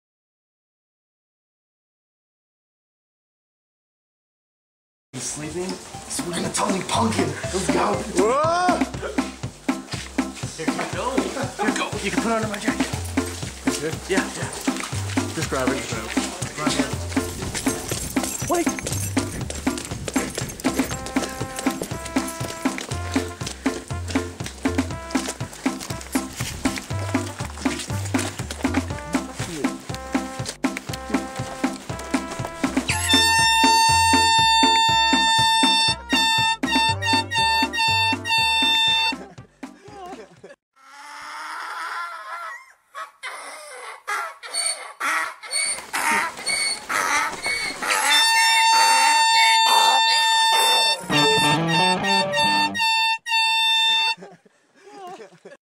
Punk'd with Skamikaze!
The fellas horsing around during recording. Nov. '09